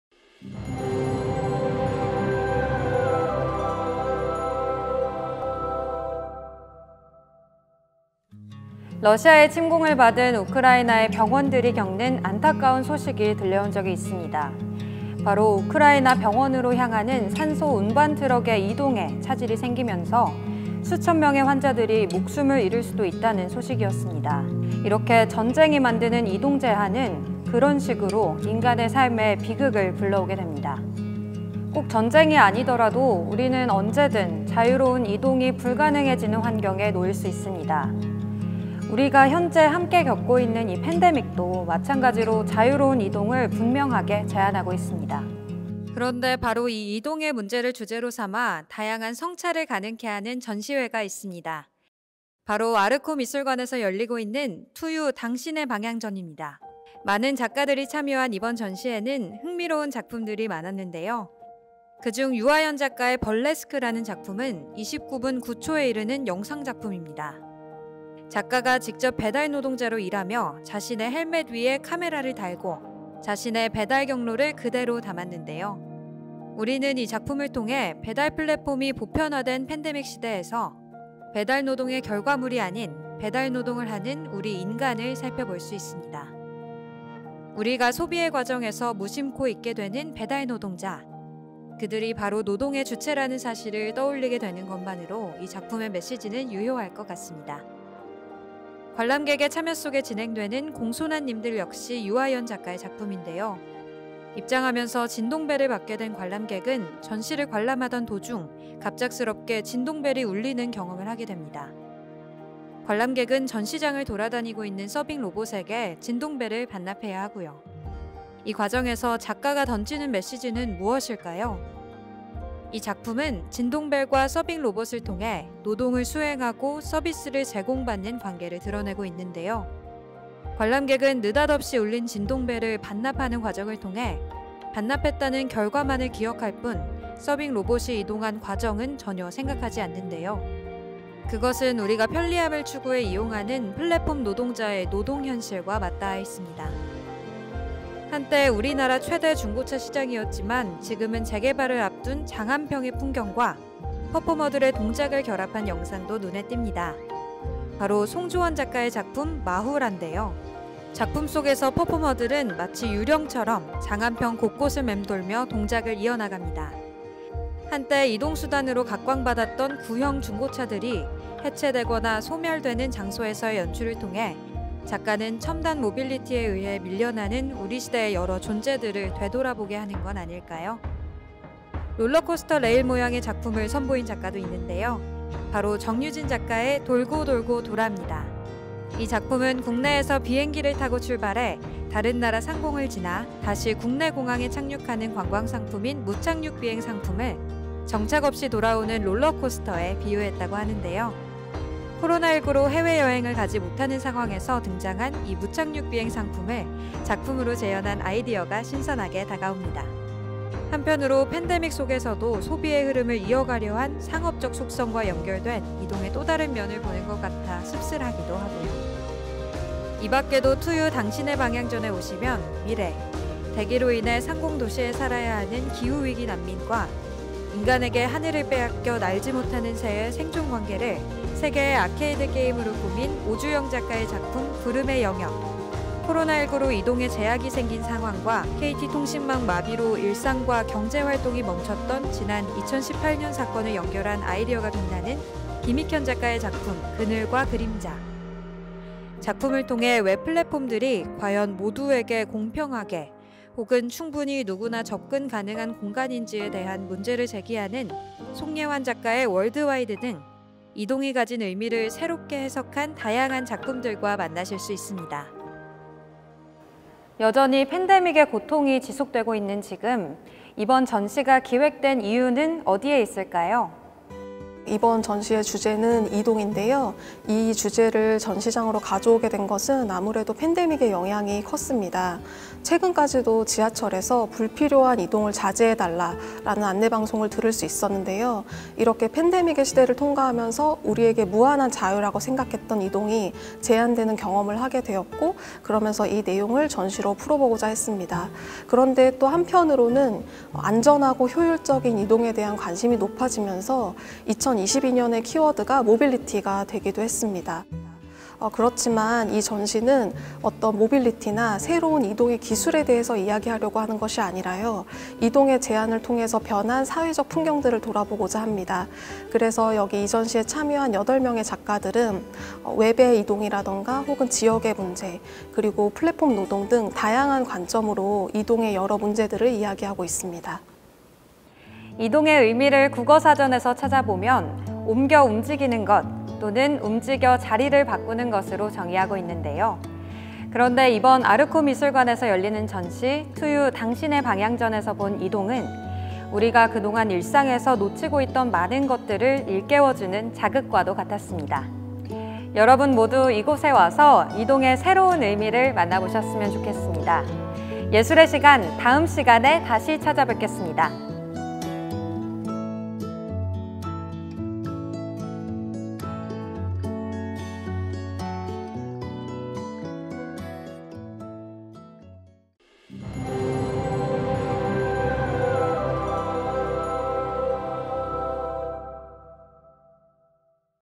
[예술의 시간] 아르코미술관 주제기획전 [투 유: 당신의 방향]
'투유 : 당신의 방향'  전시는 대학로 아르코미술관에서 오는 4월 24일까지 무료로 만나볼 수 있습니다.

자유로운 이동을 제한당하고 있는 펜데믹상황에서 ‘이동’을 주제로 내세운 흥미로운 전시회가 열리고 있습니다.

바로 아르코미술관에서 진행하는 ‘투 유: 당신의 방향’ 전. 작가가 직접 배달노동자로 일하며 헬멧 위에 카메라를 달고 배달 경로를 담아낸 영상을 통해 배달노동의 주체인 배달원을 인식케하는 ‘벌레스크’ 등 다양한 작가들의 실험적인 작품들을 만날 수 있습니다.